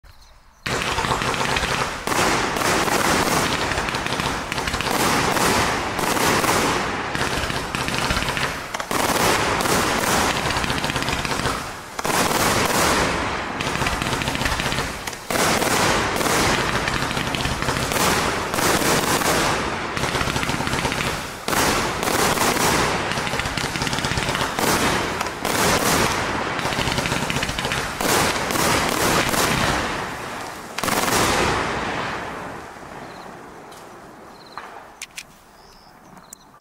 JT6157 1.2inch 100s I shape Cakes.---30secs
All with blue to red strobe with crackling chrys mine;
Break Flower crown;
Spit blue dahlia horsetail.